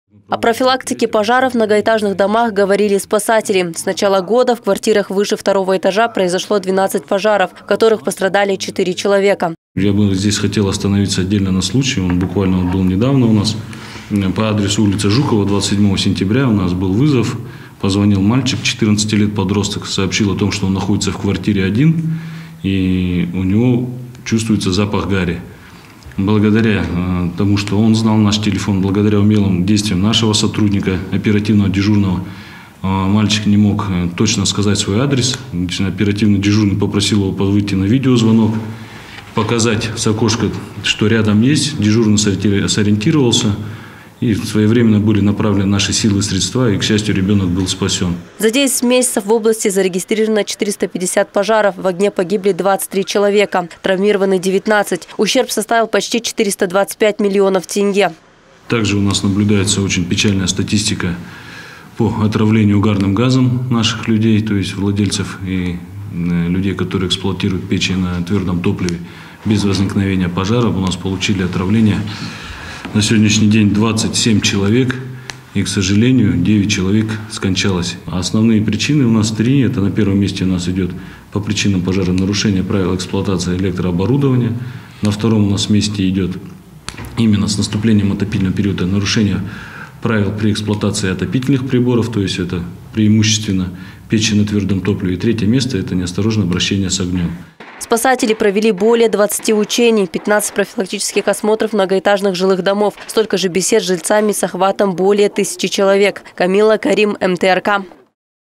С начала года в области зарегистрировано 450 пожаров
О профилактике пожаров в многоэтажных домах говорили спасатели. С начала года в квартирах выше второго этажа произошло 12 пожаров, в которых пострадали 4 человека.